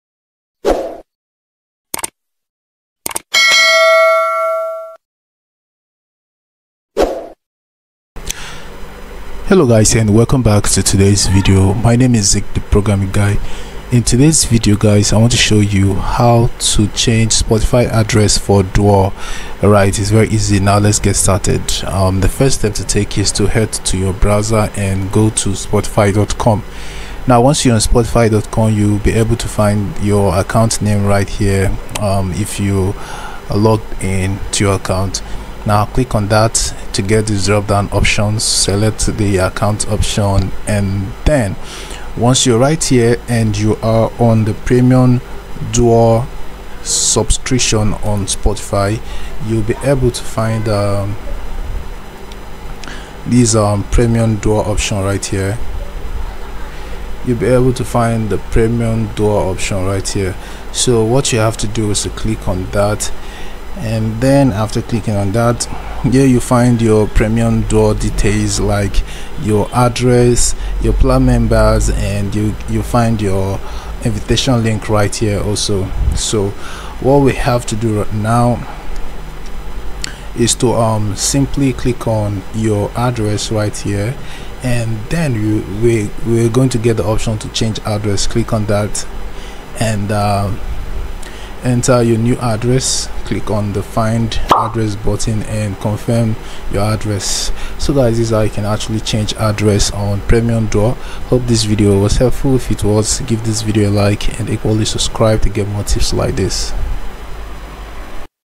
How to Change Spotify Address For Duo
Learn how to change spotify address for duo. After adding your address on spotify duo subscription, you can still change it at anytime.

This video is related to :-
► spotify duo address
► spotify duo same address
► spotify duo address not working
► change to spotify duo